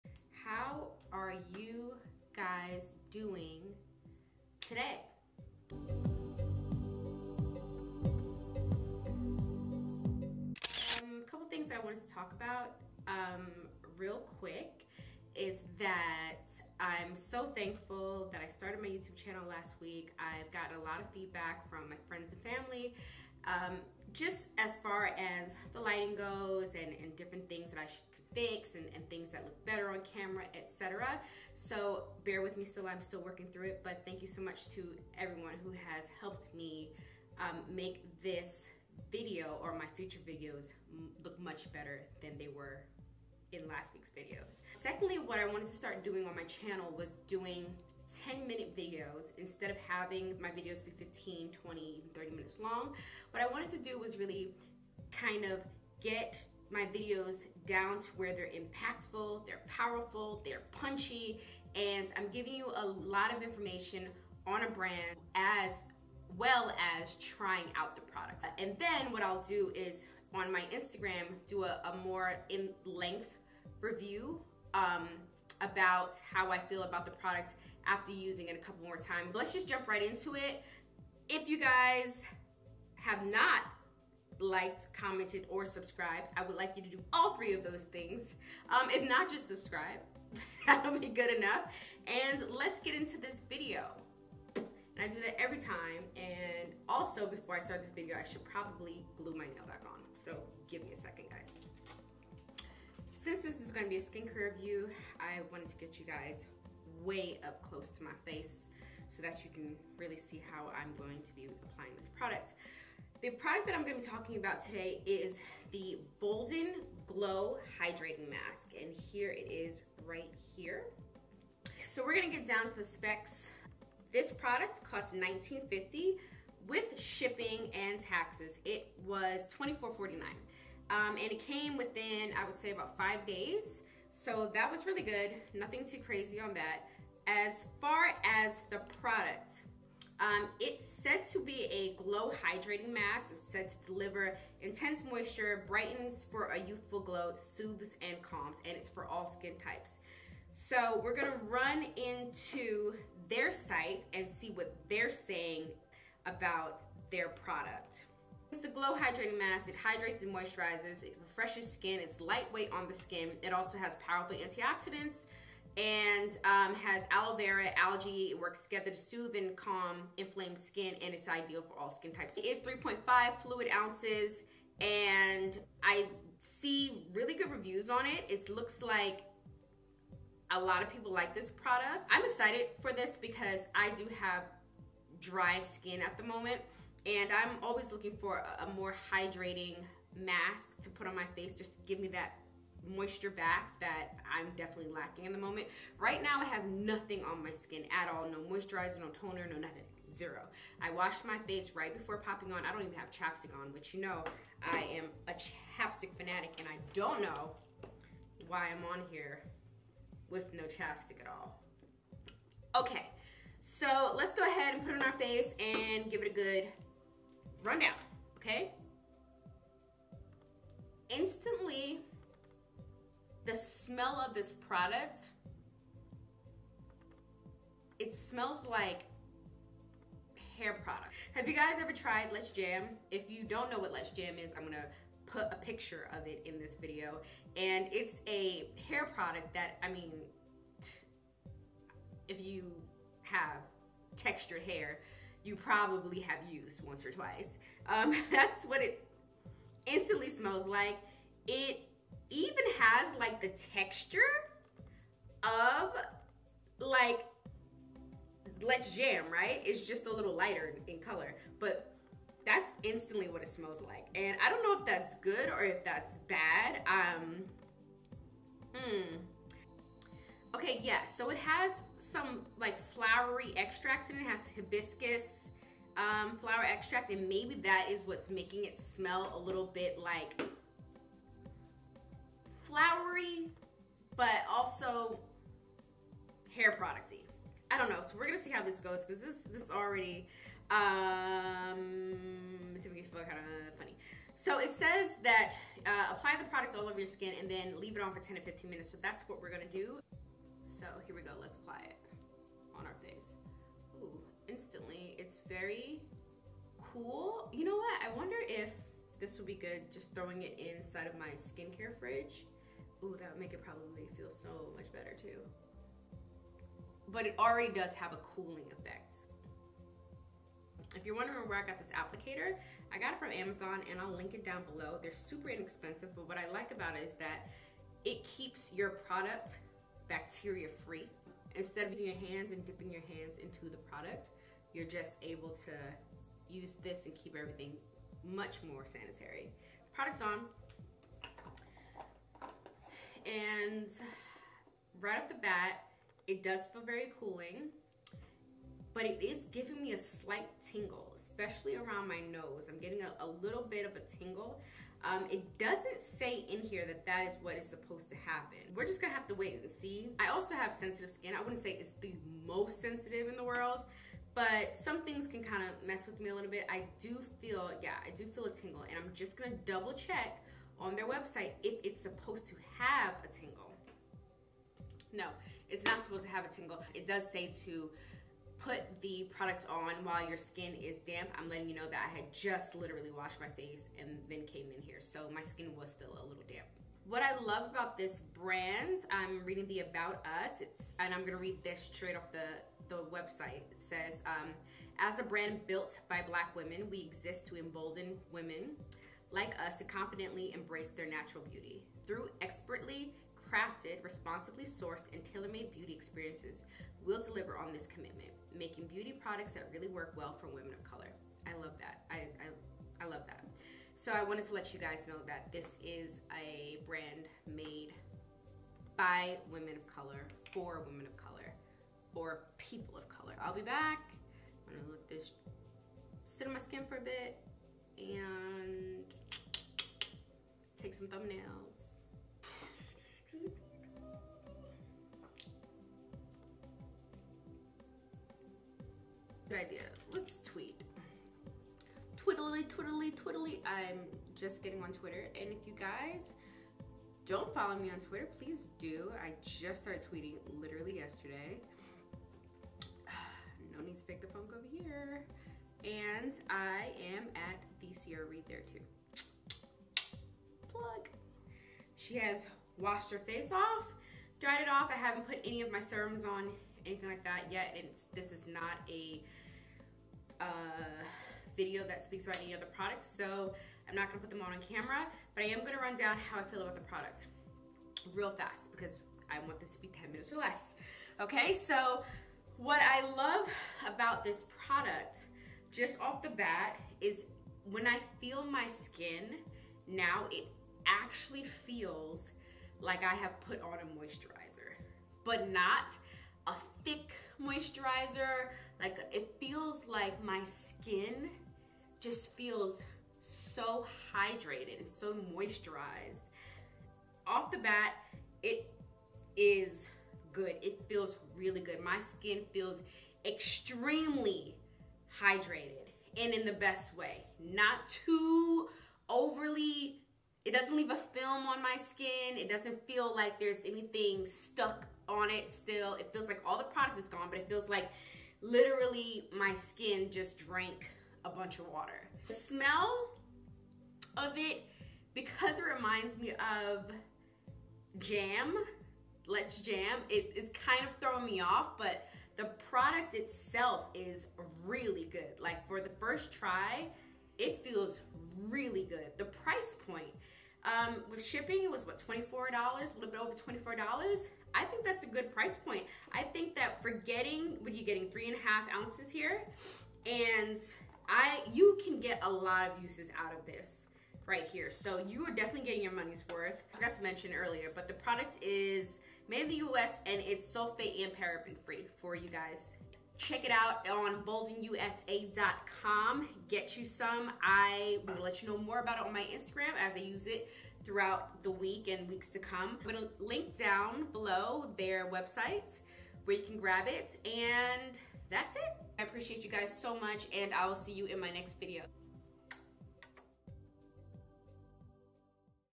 BOLDEN GLOW HYDRATING MASK REVIEW !! IS IT WORTH YOUR TIME?? FOR REAL THO? :/
HEY GUYS!
I tried using Boldens Glow Hydration Mask using my new format for videos!
As of today my videos will be only 10 Minutes or less! I am doing this because I feel that most people don't have the time to watch long videos and want to get to the point which is-  Is it worth it buying?
Let me know if you guys like this format better!
Also! MY AUDIO IS A BIT LOW IN THIS VIDEO! I watched a YouTube video and followed it EXACTLY  and well , it sucked (HAHA)
I have fixed it (hopefully) in the videos moving forward

Check out Boldens Products on :
Boldenusa.com